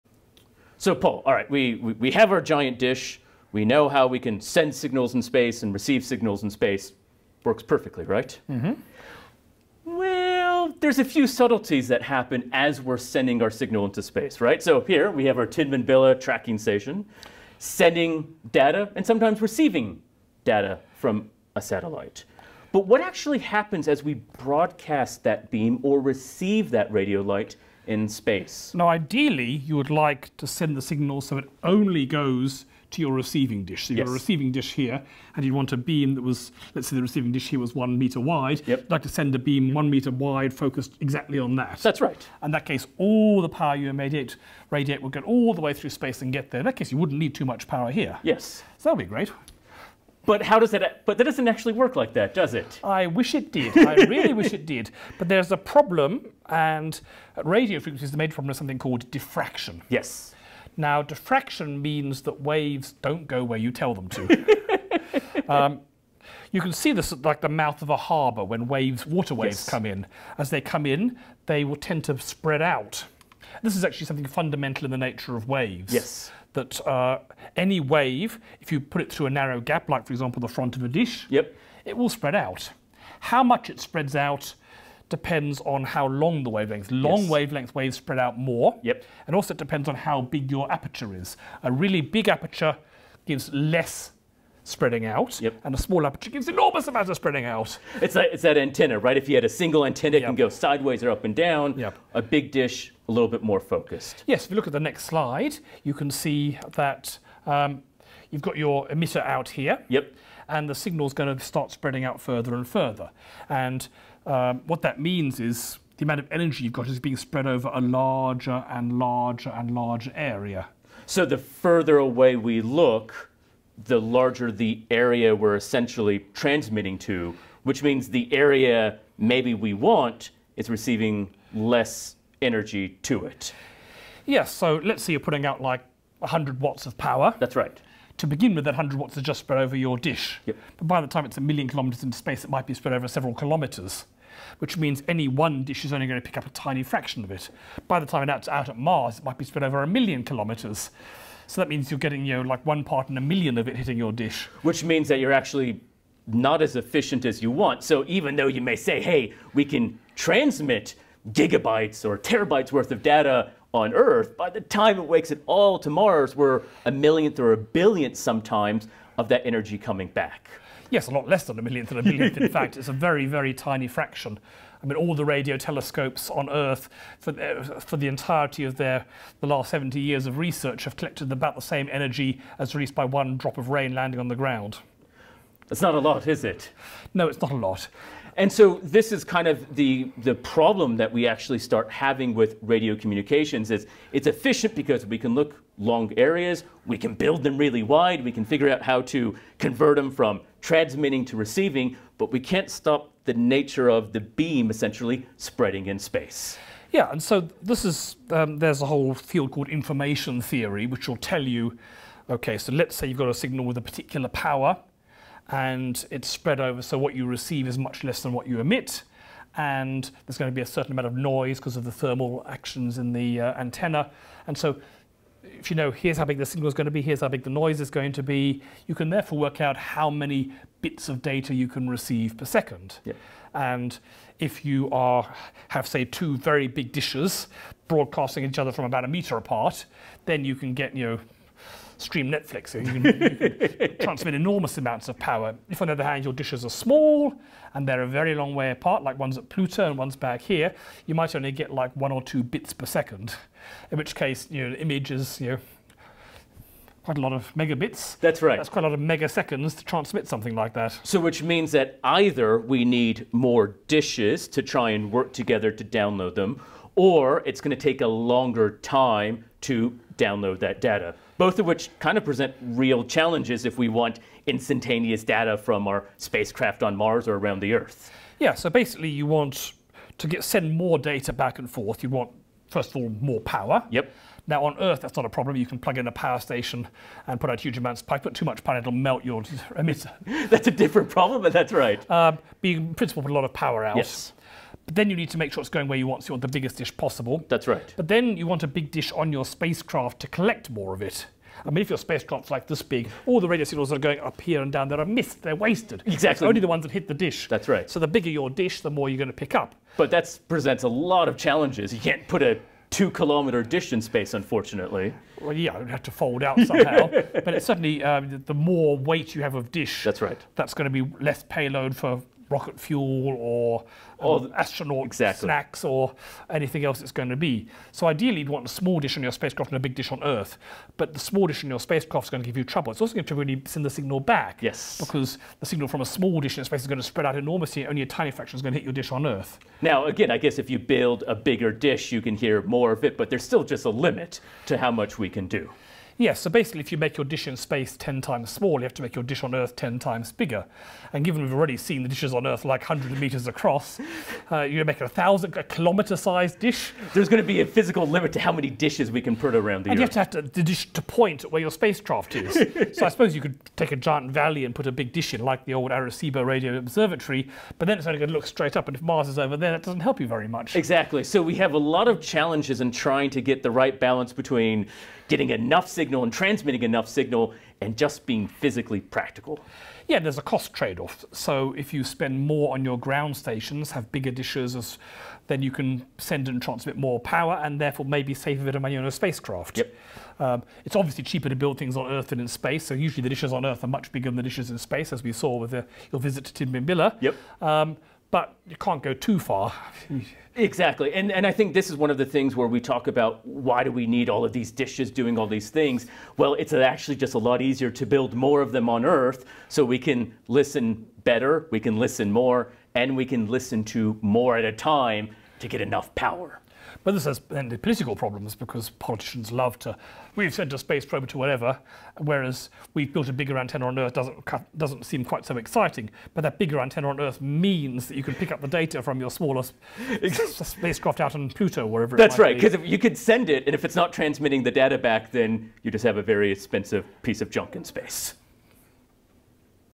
Space V8.4 Beam Divergence and the Inverse Square Law
The strength of a radio signal decreases over distance in space.